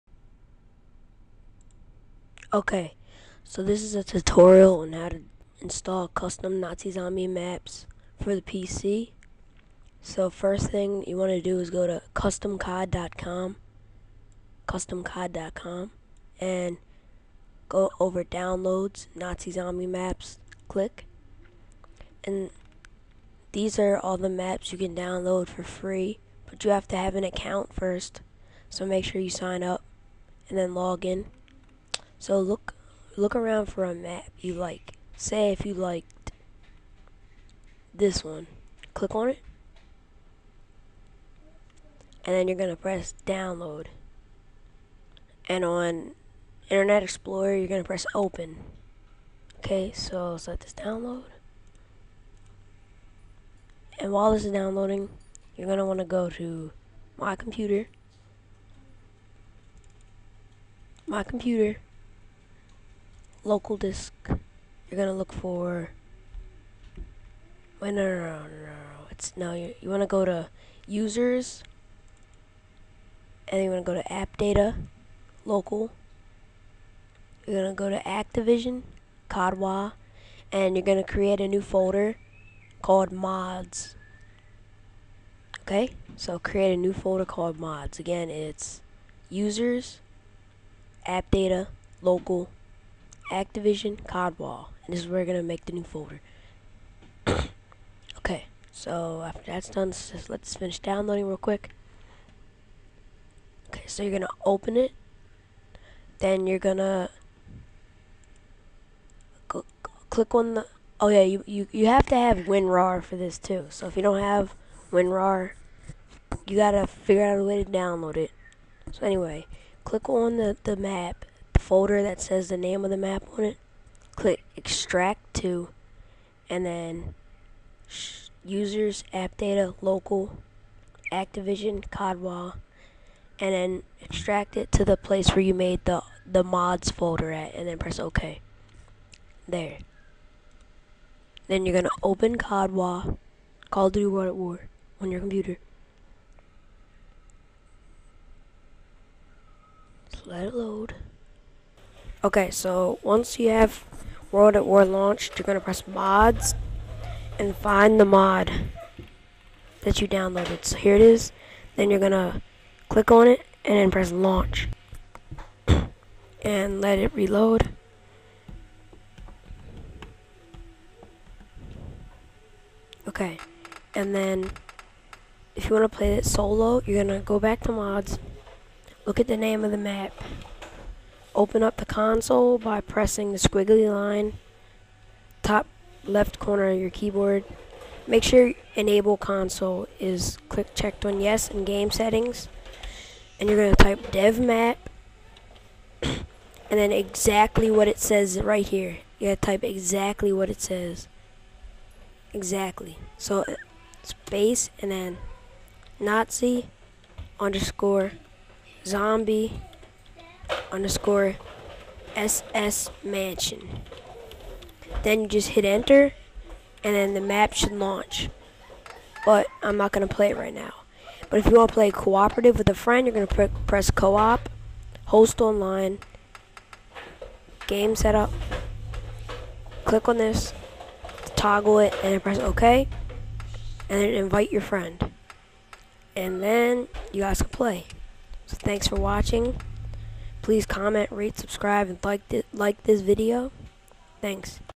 How to Install Custom Zombie Maps On PC
This video will show you guys how to download, install, and play the custom zombie maps.
Type this into the console to launch solo:
devmap (name of map)